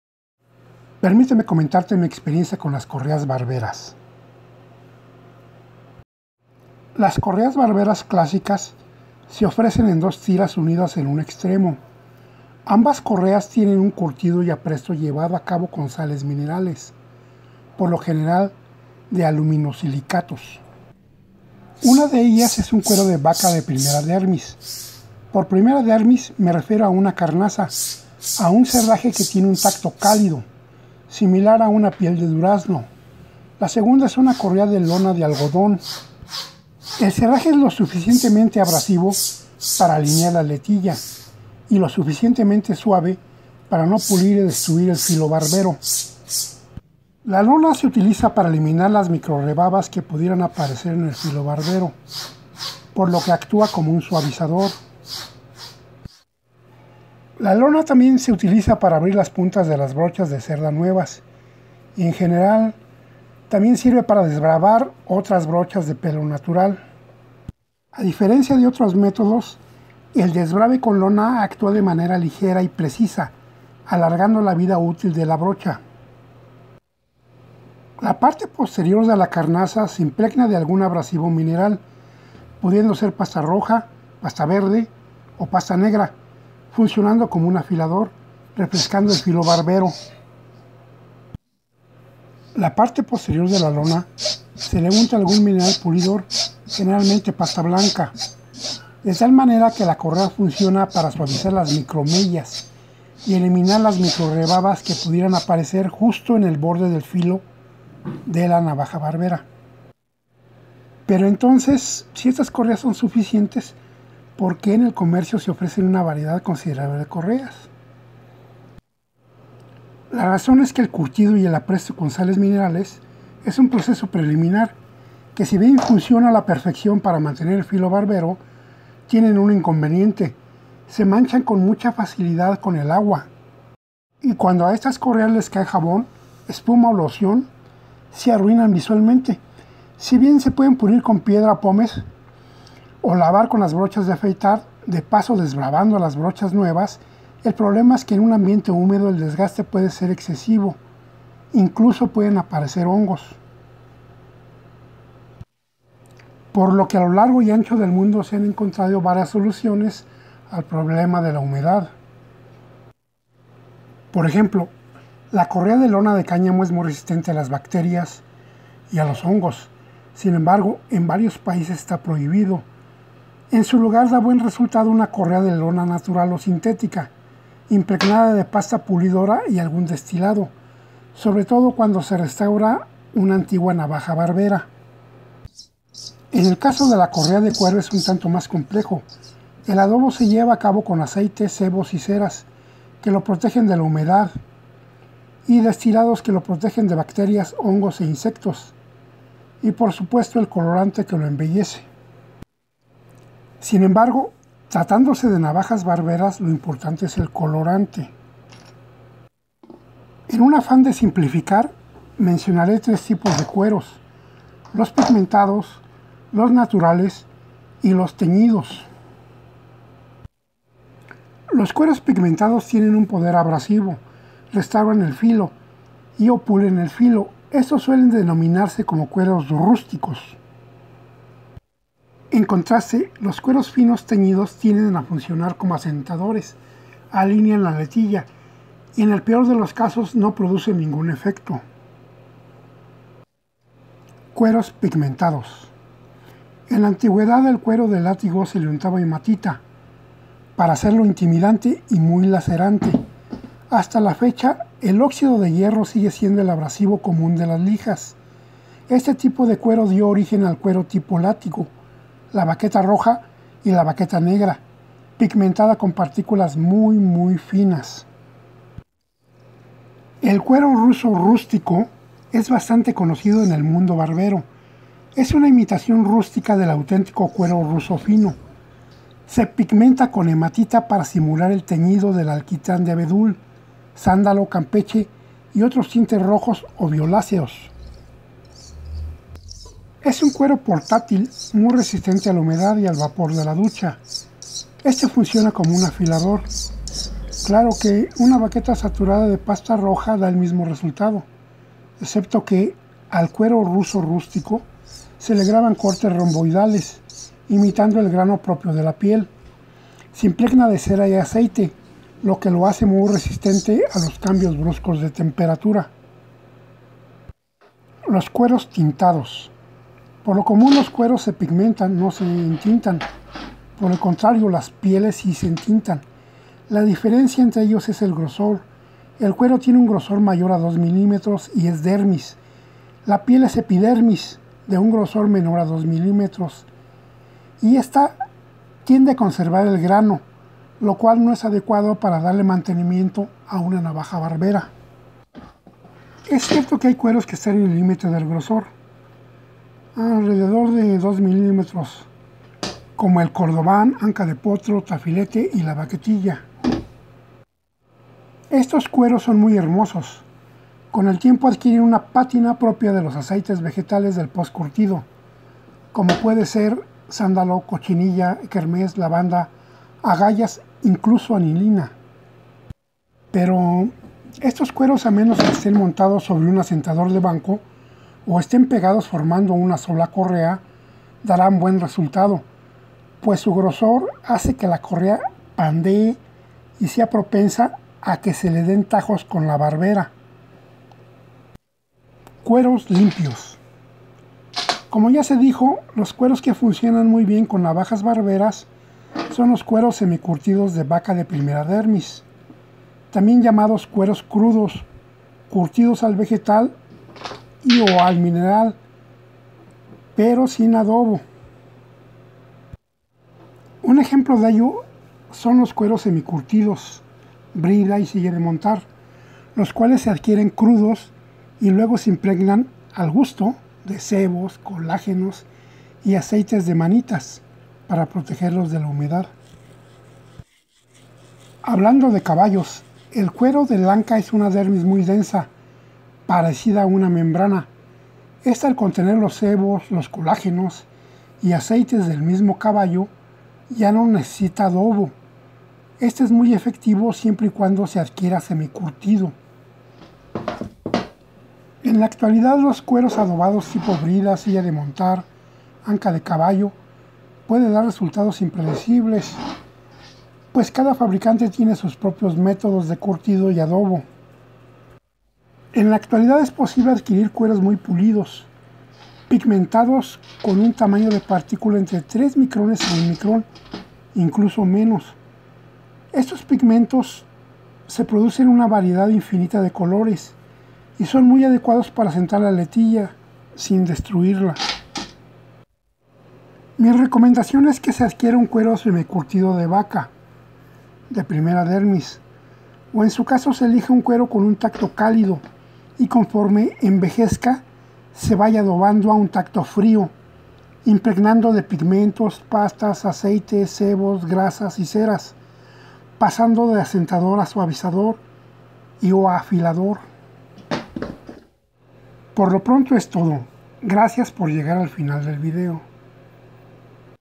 Correas Barberas, Asentador de Cuero.
Se muestra varios tipos de correas barberas: suavizador, asentador y afilador. Vaqueta, vaquetilla, carnaza, serraje, brida, silla de montar, cuero ruso, látigo, lona, anca de potro...